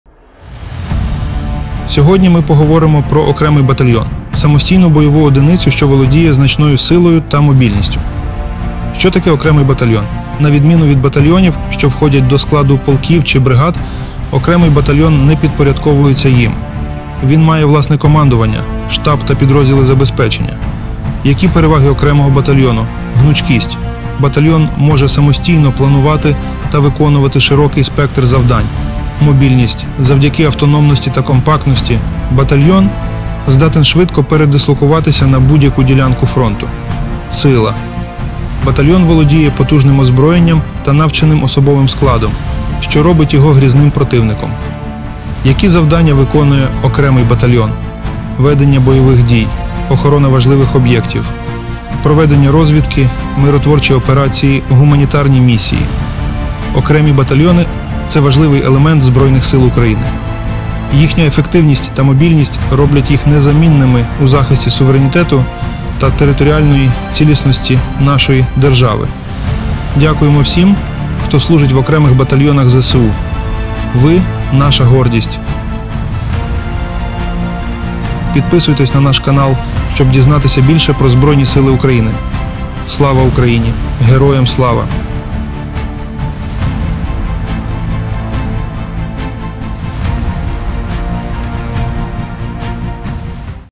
Що таке окремий батальйон?
Відкрийте для себе світ окремих батальйонів Збройних Сил України!
У цьому відео ми дослідимо цю самостійну бойову одиницю, що володіє значною силою та мобільністю.
Дізнайтеся:
Які його переваги?
Які завдання він виконує?
Чому окремі батальйони такі важливі для ЗСУ?

Рекламна агенція Артангел
Створення сайтів - artangel.in.ua
Дизайн каталогів та поліграфії
Монтаж відео для Youtube
Фото продукції та подій
Artangel.org.ua